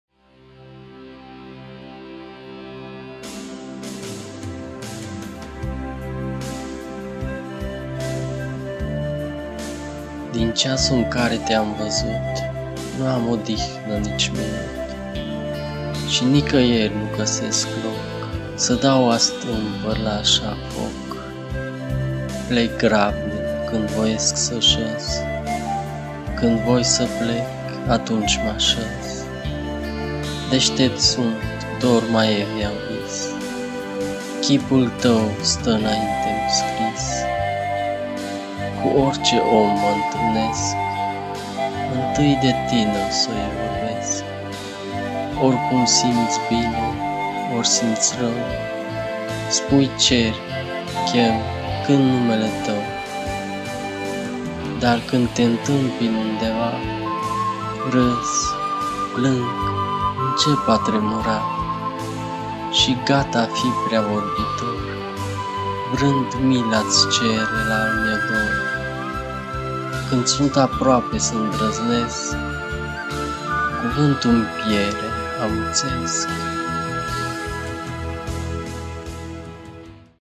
Adevărata iubire - Iancu Văcărescu
Din ceasu-n care te-am văzut,
Nu am odihnă nici minut,
Şi nicăiri nu găsesc loc
Să dau astâmpăr l-aşa foc,
Plec grabnic, când voiesc să şăz,
Când voi să plec, atunci m-aşăz.

Deştept sunt, dorm, aievea-n vis
Chipul tău stă-nainte-mi scris!
Cu orice om mă întâlnesc,
Întâi de tine-oi să-i vorbesc,
Oricum simţ bine, ori simţ rău,
Spui, cer, chem, cânt numele tău.

Dar când te-ntâmpin undeva,
Râz, plâng, încep a tremura,
Şi gat-a fi prea vorbitor,
Vrând mil' a-ţi cere l-al meu dor,
Când sunt aproape s-îndrăznesc,
Cuvântu-mi piere, amuţesc.

- voce Daniel Nemeş